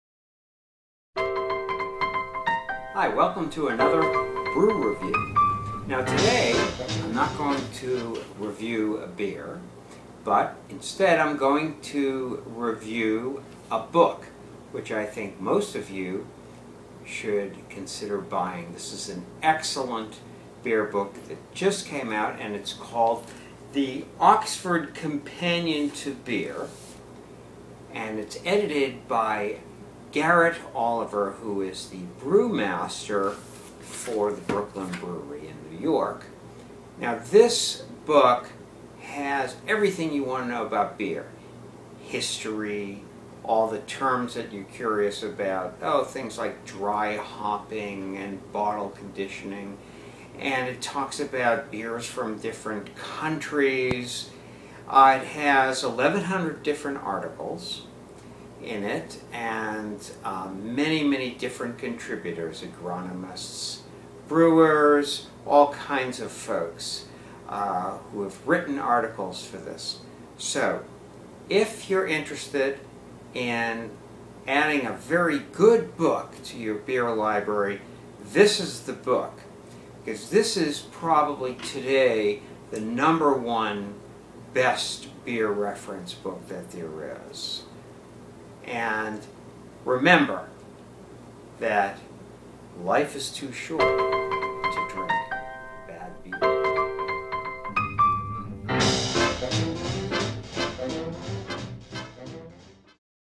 Oxford Companion to Beer edited by Garrett Oliver: The Bru Review
Bru reviews The Oxford Companion to Beer. The ultimate beer reference book.